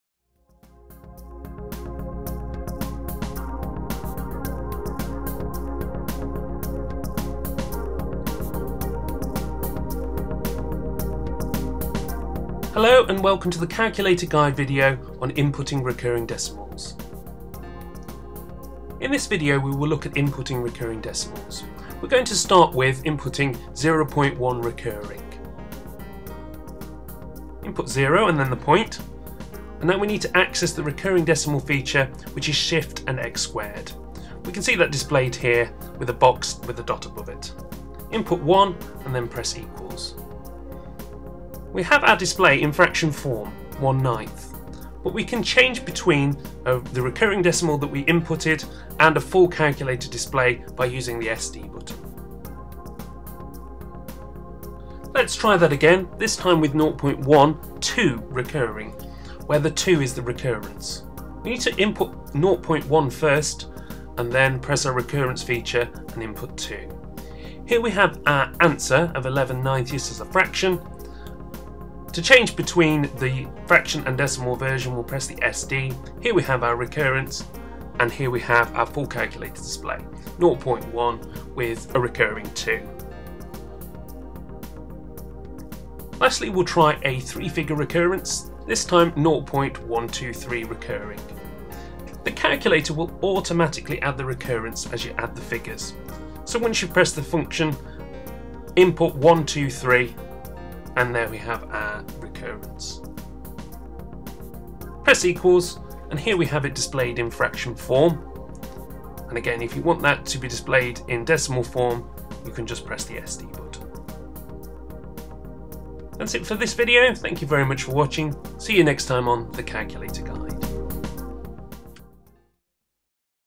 How To Input Recurring Decimals (Repeating) - Casio Calculator fx-83GT PLUS fx-85GT PLUS GCSE
⨸ Aʙᴏᴜᴛ Tʜɪs Vɪᴅᴇᴏ ⨸  - How To Input Recurring or Repeating Decimals.
This video shows you how to use the Casio calculator to input recurring (repeating) decimals and how to switch between a fraction and decimal display. On certain models, this will be shown with a bar across the top rather than a dot.